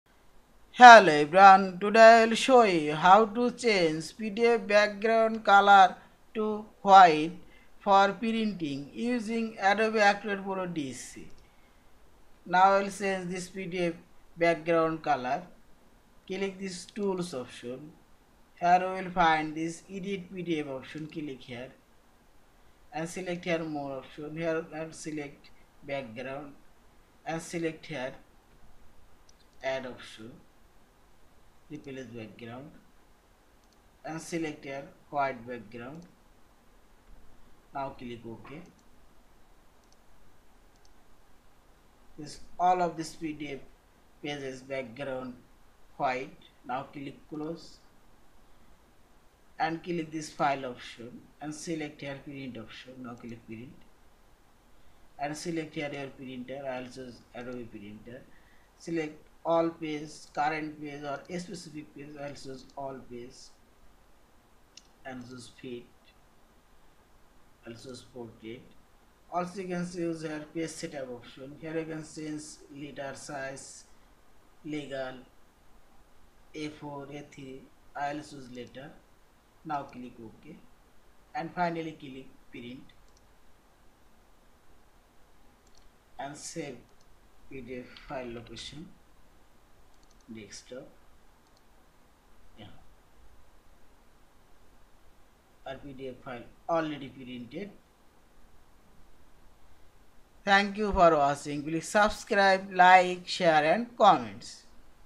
How to change pdf background color to white for printing using Adobe Acrobat Pro DC
Hello everyone, Today I will show you. How to change pdf background color to white for printing using Adobe Acrobat Pro DC.